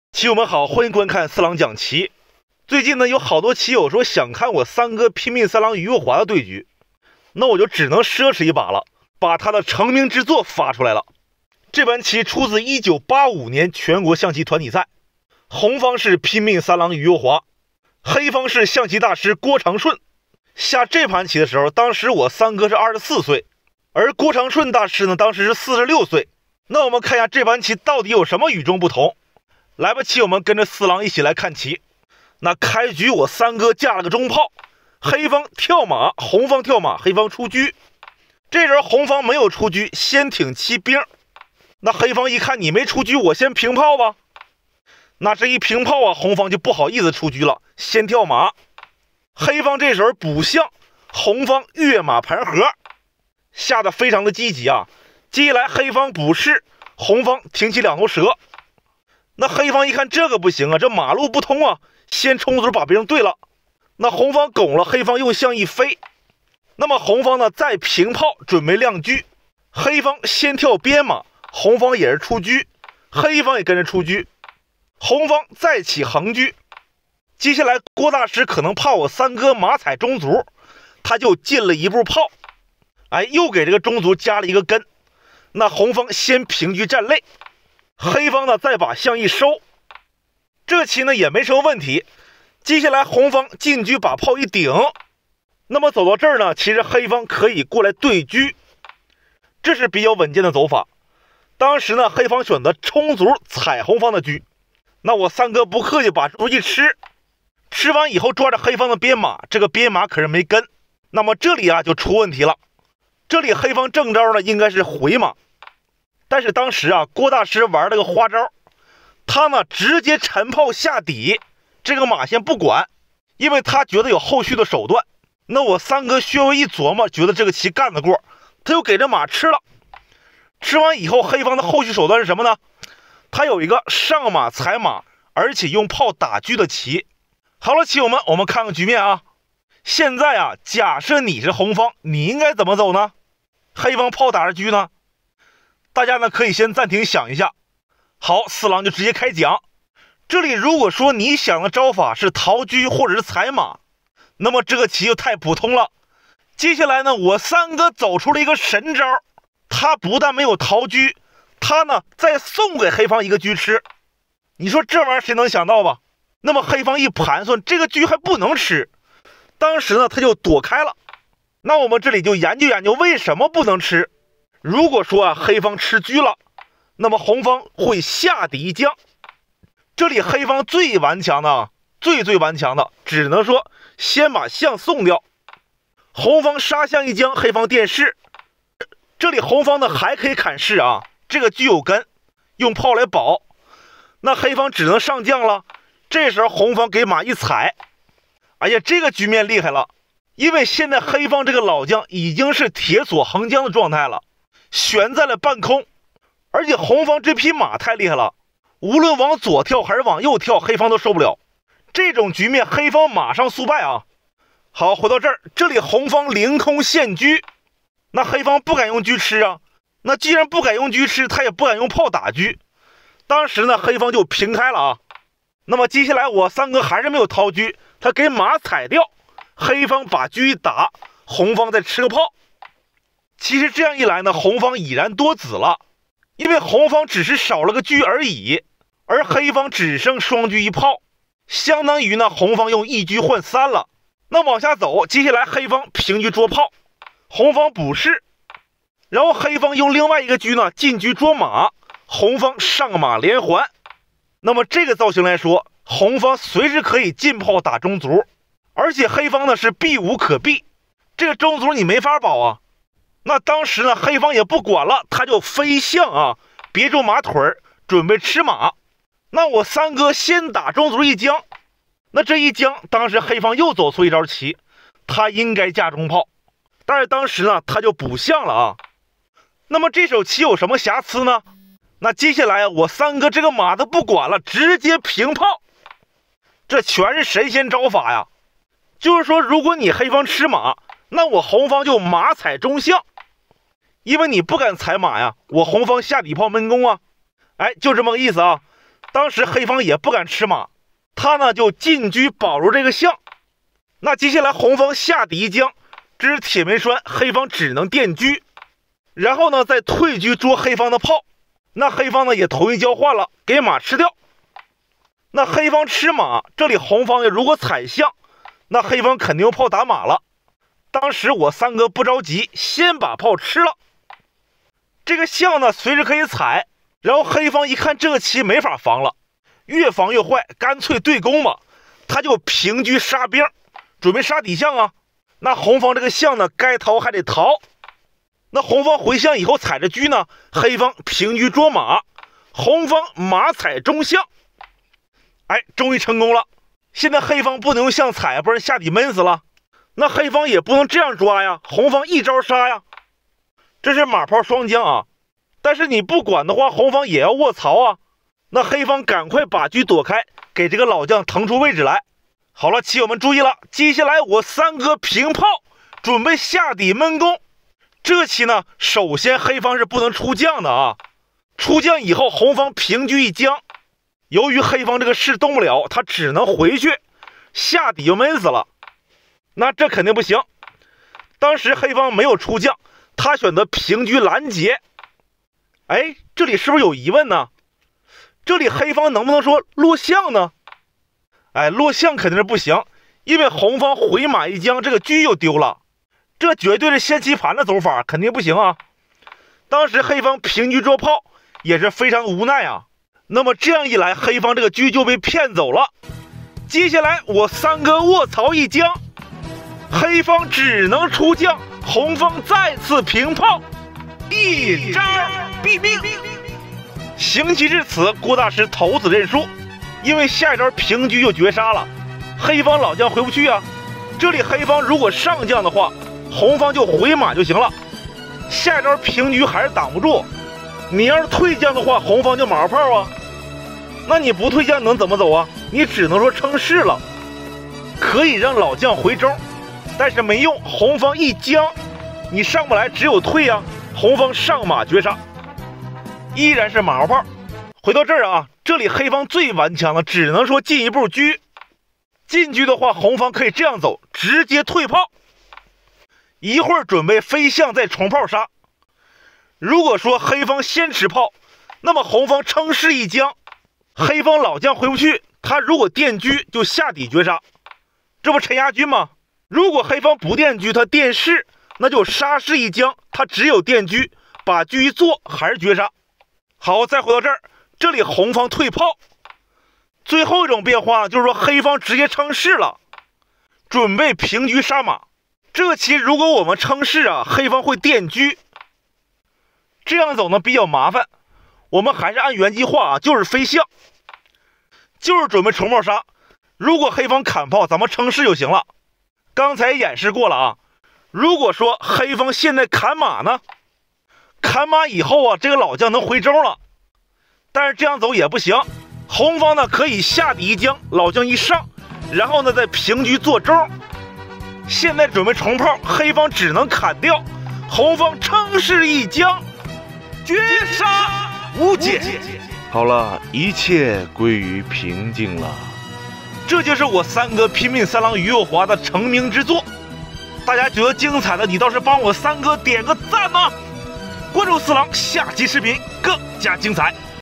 于幼华VS郭长顺，犹如天上降魔主，真是人间太岁神！绝了！【四郎讲棋】
各位棋友，大家好，欢迎来到四郎讲棋。本期为大家带来的是于幼华与郭长顺的经典名局，拼命三郎上来直接上大招，太厉害了！
于幼华 郭长顺 象棋对战 请注意，本视频完全符合YouTube社区准则，没有任何违规行为。